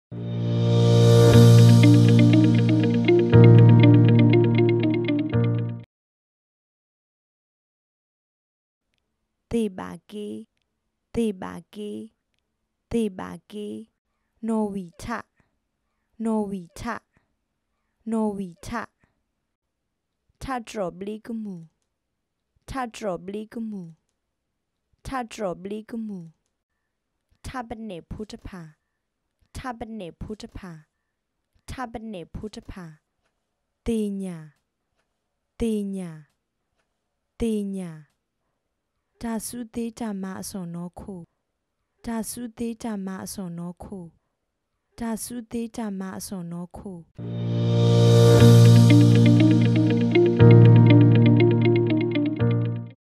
Karen Vocabulary Episode 41 Karen Workshop Part 2
Welcome to Karen Center.Karen Vocabulary Episode 40 this video will teach you how to call Screwdriver, Plank, Shavings, Wood , Nails and Workbench in karen language. ​